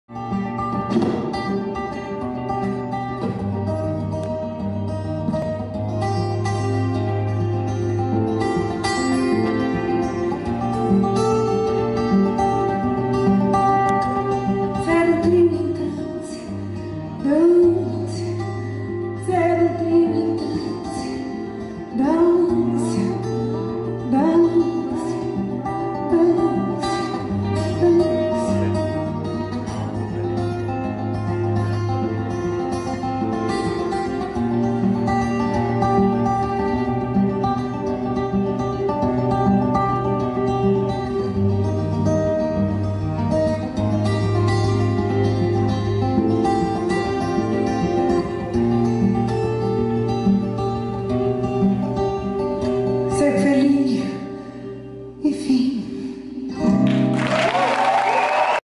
Daniela Mercury dançando (Teatro J. Safra) São Paulo
Daniela Mercury mostrando a sua formação em dança no show Voz & Violão que realizou em São Paulo, no Teatro J. Safra.